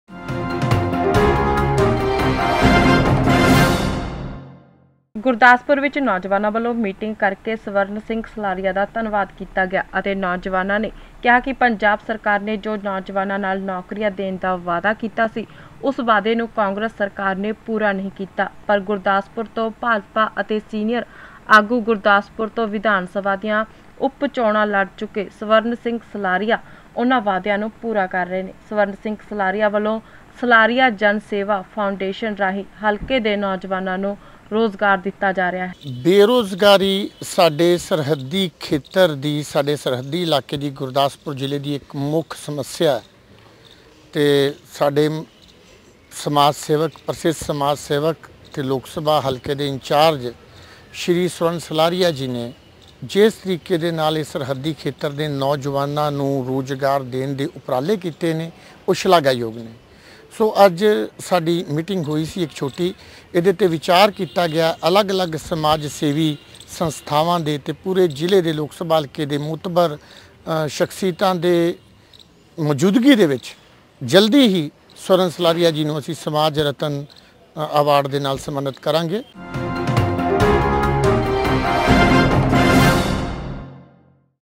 Aone Punjabi News | ਗੁਰਦਾਸਪੁਰ ਦੇ ਨੋਜਵਾਨਾਂ ਨੇ ਕੀਤਾ ਸਵਰਨ ਸਲਾਰੀਆ ਦਾ ਧੰਨਵਾਦ
ਗੁਰਦਾਸਪੁਰ ਵਿੱਚ ਨੌਜਵਾਨਾਂ ਵਲੋਂ ਮੀਟਿੰਗ ਕਰ ਕੇ ਸਵਰਨ ਸਿੰਘ ਸਲਾਰੀਆਂ ਦਾ ਧੰਨਵਾਦ ਕੀਤਾ ਗਿਆ ਅਤੇ ਨੌਜਵਾਨਾਂ ਨੇ ਕਿਹਾ ਕਿ ਪੰਜਾਬ ਸਰਕਾਰ ਨੇ ਜੋ ਨੌਜਵਾਨਾਂ ਨਾਲ ਨੌਕਰੀਆਂ ਦੇਣ ਦਾ ਵਾਧਾ ਕੀਤਾ ਸੀ ਉੱਸ ਵਾਧੇ ਨੂੰ ਕਾਂਗਰਸ ਸਰਕਾਰ ਨੇ ਪੁਰਾ ਨਹੀਂ ਕੀਤਾ ਲੇਕਿਨ ਗੁਰਦਾਸਪੁਰ ਤੋਂ ਭਾਜਪਾ ਦੇ ਸੀਨੀਅਰ ਆਗੂ ਅਤੇ ਗੁਰਦਾਸਪੁਰ ਤੋਂ ਵਿਧਾਨ ਸਭਾ ਦੀਆਂ ਉਪ ਚੋਣਾਂ ਲੜ ਚੁੱਕੇ ਸਵਰਨ ਸਿੰਘ ਸਲਾਰੀਆਂ ਉਹਨਾਂ ਵਾਅਦਿਆਂ ਨੂੰ ਪੂਰਾ ਕਰ ਰਹੇ ਹਨ ਸਵਰਨ ਸਿੰਘ ਸਲਾਰੀਆਂ ਵਲੋਂ ਸਲਾਰੀਆਂ ਜਨ ਸੇਵਾ ਫਾਉਂਡੇਸ਼ਨ ਰਾਹੀਂ ਹਲਕੇ ਦੇ ਨੌਜਵਾਨਾਂ ਨੂੰ ਰੋਜ਼ਗਾਰ ਦਿੱਤਾ ਜਾ ਰਿਹਾ ਹੈ ਜੋ ਕਿ ਇਸ ਤਰ੍ਹਾਂ ਦਾ ਉਪਰਾਲਾ ਪੰਜਾਬ ਸਰਕਾਰ ਨੂੰ ਕਰਨਾ ਚਾਹੀਦਾ ਸੀ ਸਵਰਨ ਸਿੰਘ ਸਲਾਰੀਆਂ ਦੇ ਇਸ ਉਪਰਾਲੇ ਤੋਂ ਹਲਕੇ ਦੇ ਨੋਜਵਾਨ ਕਾਫੀ ਖੁਸ਼ ਹਨ ਅਤੇ ਇਸ ਉਪਰਾਲੇ ਲਈ ਨੌਜਵਾਨਾਂ ਵਲੋਂ ਕਿਹਾ ਗਿਆ ਕਿ ਉਹ ਸਵਰਨ ਸਿੰਘ ਸਲਾਰੀਆਂ ਨੂੰ ਜਲਦ ਹੀ ਇਕ ਐਵਾਰਡ ਦੇ ਕੇ ਸਨਮਾਨਿਤ ਕਰਨਗੇ

Download

channel

What'saap no.94784-84087

Download

channel